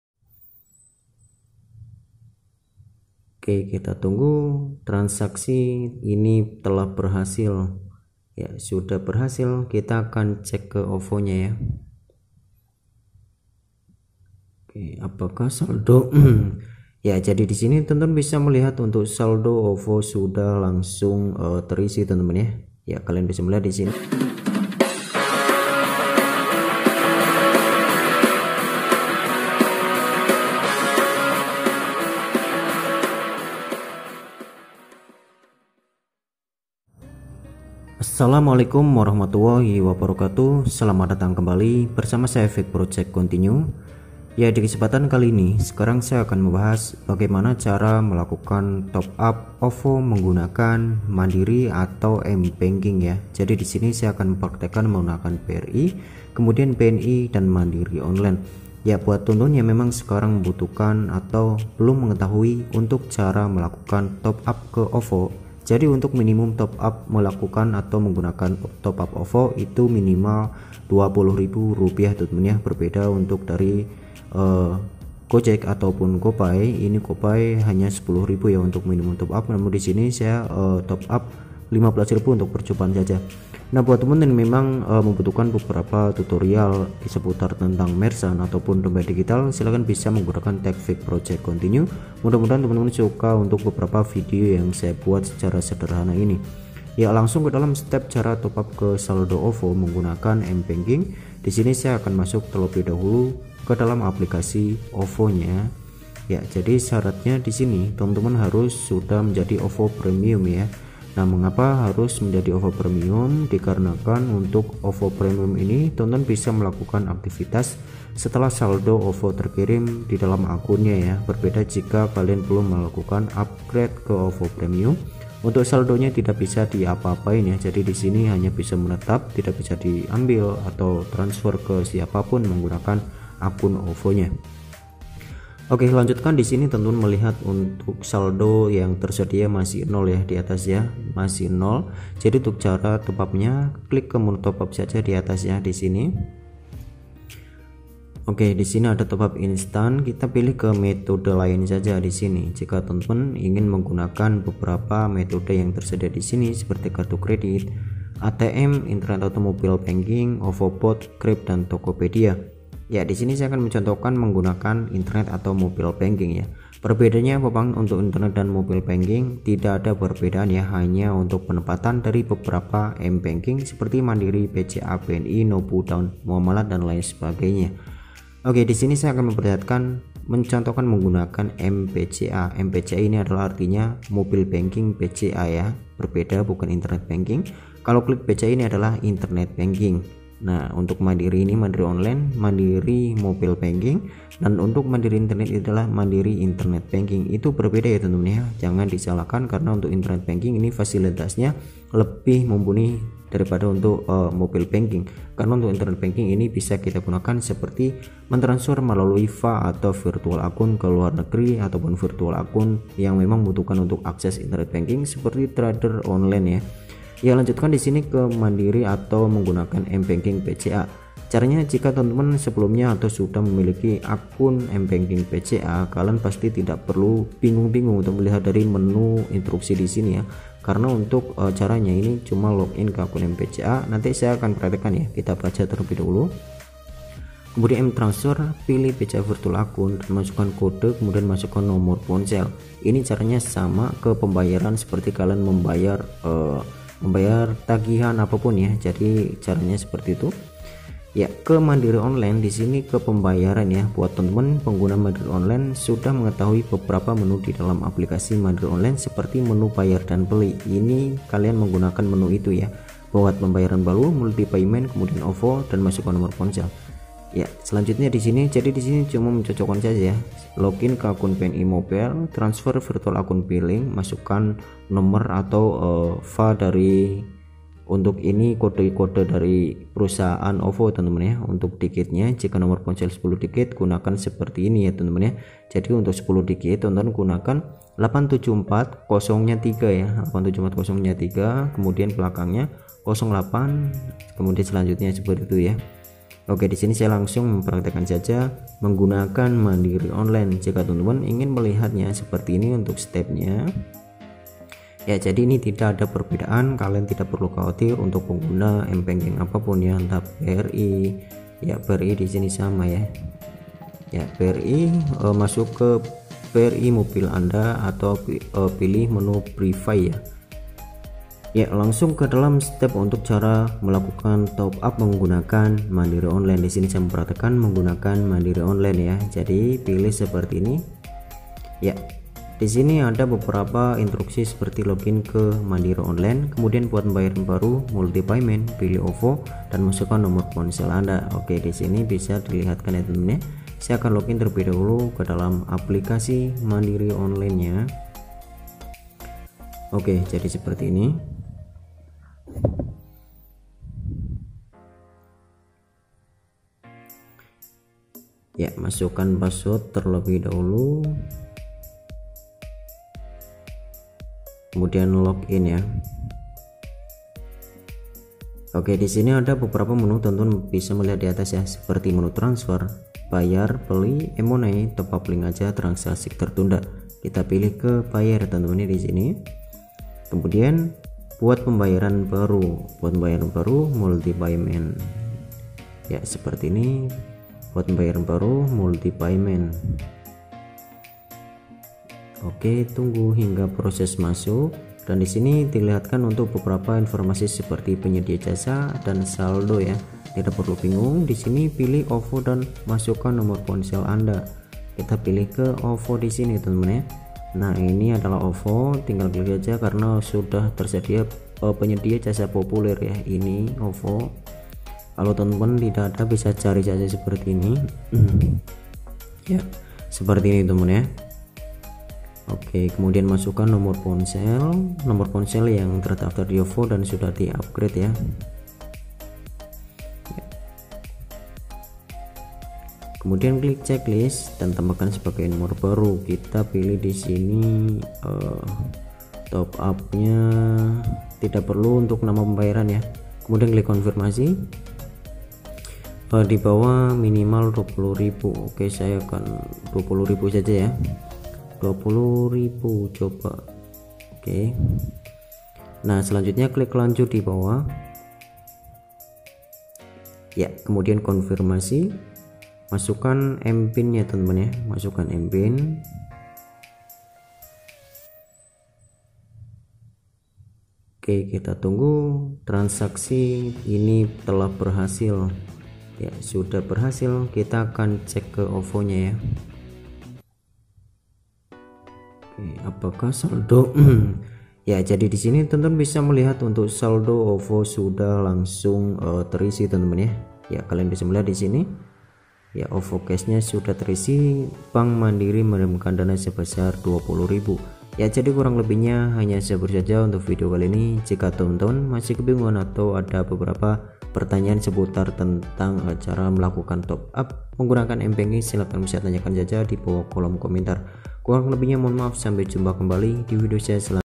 Tanpa Ribet ! Cara Topup OVO Pakai Internet / Mobile Banking ( Trial : Mandiri Online )
cara top up ovo grab via bni mobile banking,